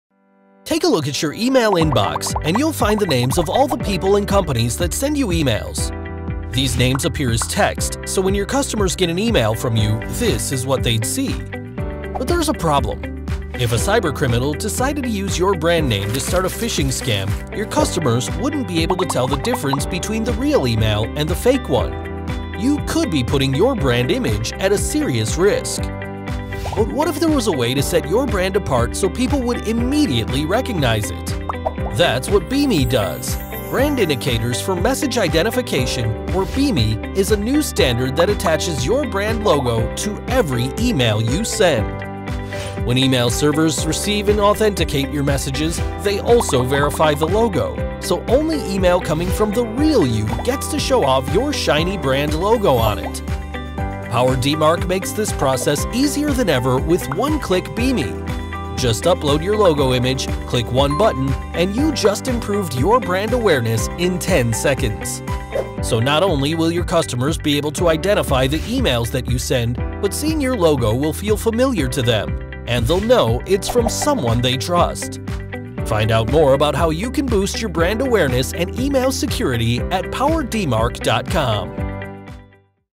What is BIMI (Brand Indicators for Message Identification)?
🔍Learn the essentials of Brand Indicators for Message Identification (BIMI) in our video. Learn What is BIMI and how it enhances cybersecurity by enabling verified brands to display their BIMI logo directly in users' inboxes.

BIMI boosts brand trust, improves email security, and complements email authentication protocols like DMARC to keep your communications safe. Perfect for anyone interested in the intersection of cybersecurity and brand integrity. 🌐✨

Timecamp:
0:00 - Email Imbox
0:15 - Possible Phishing Scams
0:32 - What BIMI Does
0:53 -  BIMI with PowerDMARC